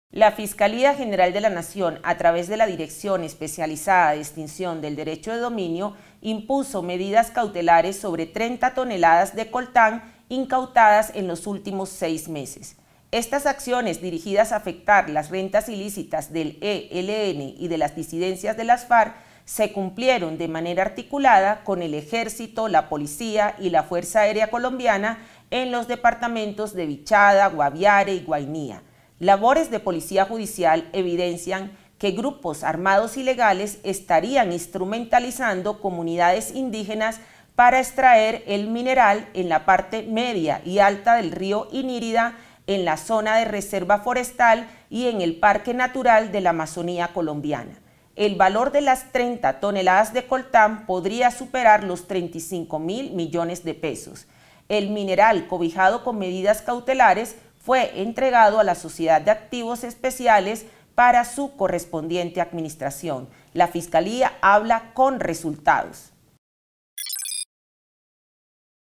Fiscalía impone medidas cautelares con fines de extinción de dominio a 30 toneladas de coltán
Las acciones operativas se realizaron en Vichada, Guaviare y Guainía; y han estado dirigidas a afectar las rentas ilícitas de las disidencias de las FARC y el ELN. Las 30 toneladas de coltán podrían superar los 35.000 millones de pesos.

En las investigaciones se constató que estos grupos armados, al parecer, instrumentalizan a las comunidades indígenas para extraer el mineral en la parte media y alta del río Inírida, en la zona de reserva forestal Puinawai y en Parques Nacionales Naturales de la Amazonía.